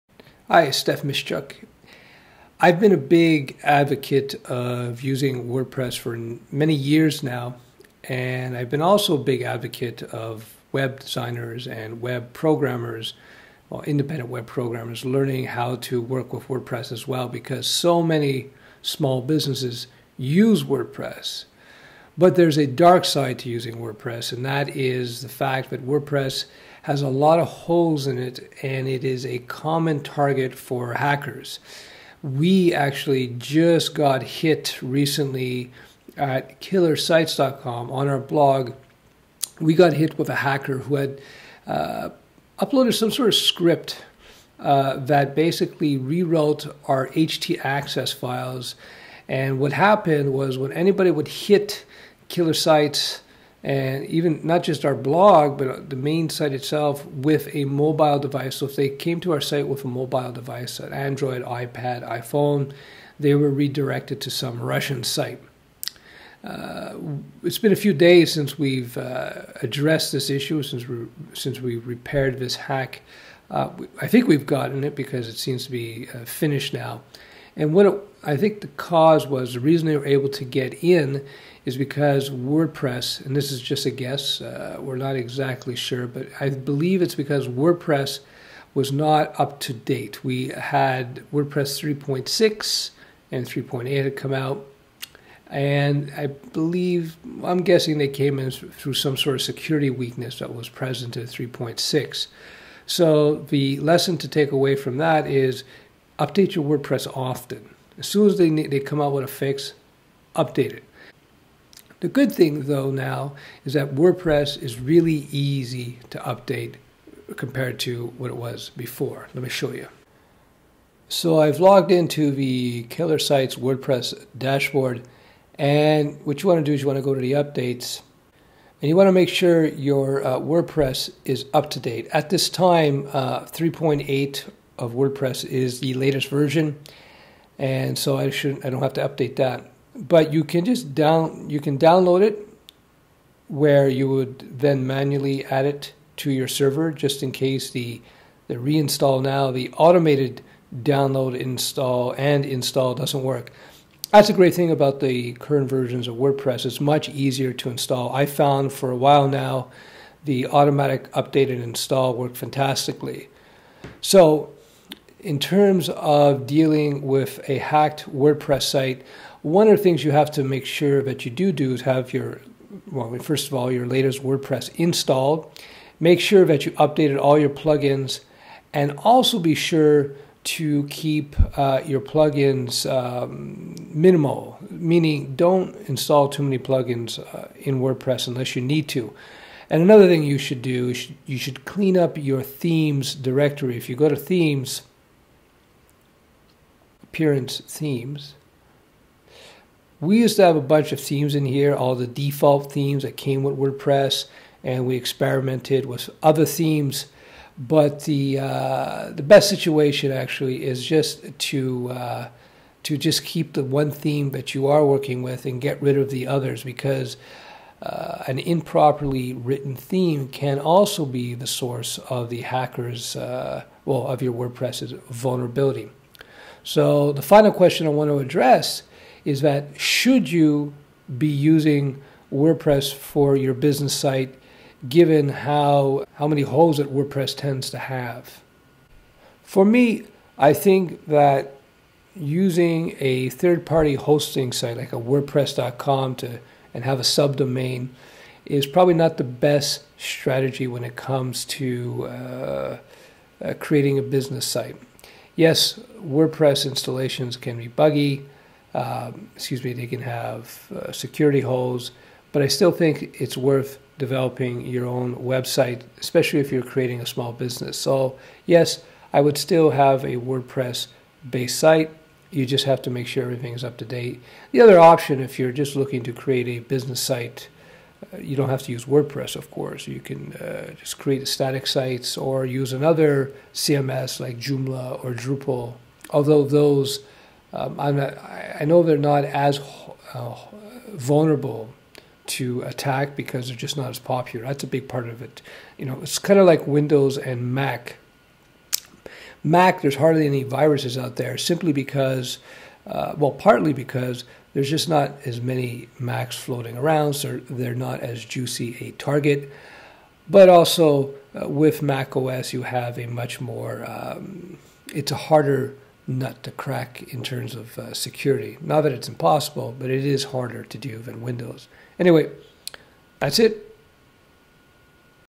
Protecting Wordpress from Being Hacked.
Hi! We were recently hacked because we were too lazy in keeping Wordpress up to date!